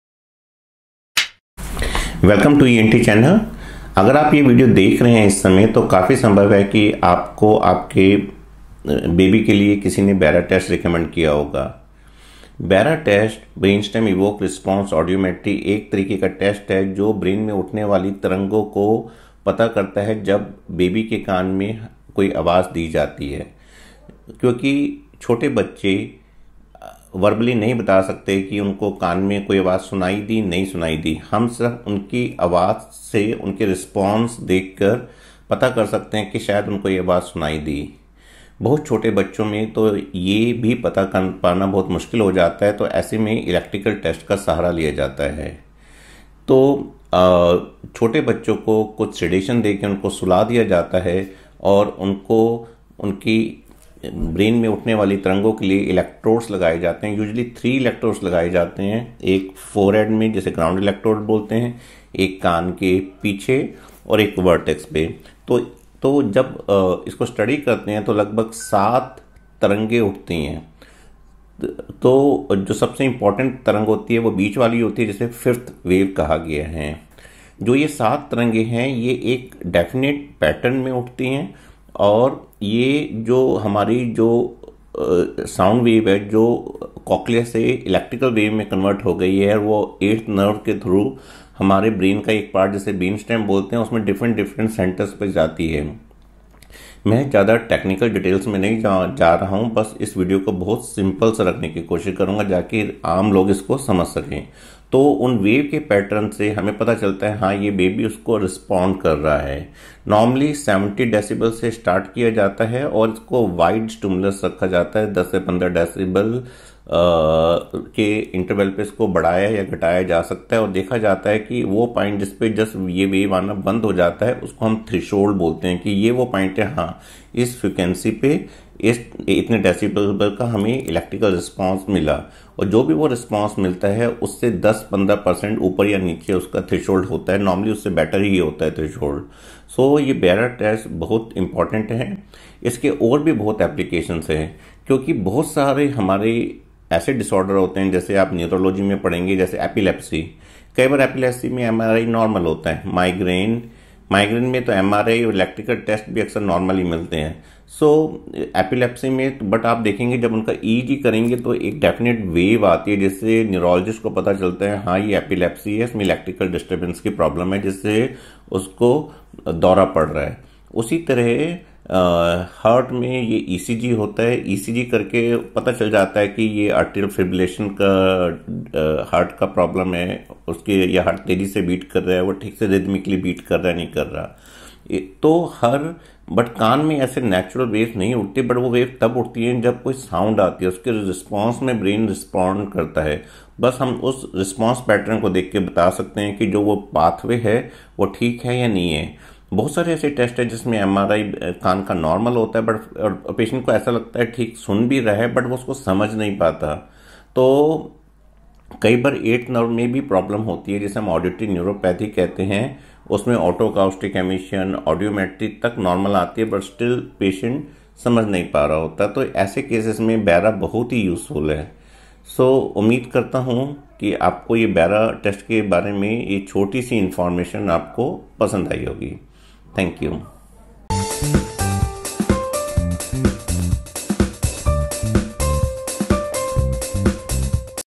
BERA Test। Newborn hearing test I नवजात बच्चों की सुनाई जाँच।
BERA Test or Brainstem Evoked Response Audiometry or ABR test or BAER test or ASSR test is a type of test which traces the electrical impulse traveling in the brain in response to a sound stimulus. This is used for testing hearing for very small children, difficult to test patients, Malingering cases of hearing, and for neuro diagnosis.

Since Little babies can't tell if they have listened to these sounds, we can tell only after seeing their response to sounds but these are not very reliable sometimes, then these electrical tests become handy.

Very small babies are made to sleep with oral sedation.  Three surface electrodes are used to capture electrical waves. One is on the forehead (Ground Electrode), the second is behind the pinna and the third is on the vertex of the head.

when we study the waves, then a total of seven peaks are observed on electrical waveforms.

The most important wave in the middle is the fifth wave or V wave.

These seven waves follow a definite pattern and these waves have been originated in the cochlea by converting mechanical sound waves into an electrical wave that travels via the VIIIth cranial nerve into many centers of the brainstem to finally approach the auditory cortex in the brain.

I don't wish to go into technical details or terms and just wish to keep this video simple for laymen to understand.

so, the pattern of these waves tells us that the patient is responding to sound stimulus.

Normally, the stimulus is started at 70 dB and it is a wide click stimulus at a frequency of 11-25 clicks per second and it is decreased or increased at an interval of 10 dB.

And the level at which wave V stops appearing is known as the hearing threshold.

whatever threshold value, we get, the corrected hearing decibel is within 10 dB of this value.

So, the BERA test is very important. There are many other applications of the BERA test.

There are many disorders in the body like in Neurology, take for example Epilepsy, Most of the time, Epilepsy have normal MRI. another example, Migraine, where MRI and Electrical tests may be normal,

But in Epilepsy, EEG may show some waveform disturbance responsible for the involuntary rhythmic movement of body parts and neurologists can make a diagnosis on basis of EEG.

Like this, ECG for the heart may detect Atrial Fibrillation (an Electrical disorder of the heart, where the heartbeat in a random way very fast).

 But in the ear, waves are produced in the brain in response to a sound stimulus.

by studying these waves, we can tell whether there is a lesion in these auditory pathways or not.

There is another group of diseases, where a patient's MRI, Audiometry, or even otoacoustic emissions are normal yet the patient is not able to hear.

these are cases where the problem is in the Auditory or VIII nerve or higher and this disorder of hearing is known as Auditory Neuropathy. These cases are best diagnosed with help of the BERA test.

I hope that you would have liked this small quick information about the BERA test.

Timestamps

00:00 Introduction
00:53 Procedure during BERA test
01:10 Study of Waveforms
03:20 Auditory Neuropathy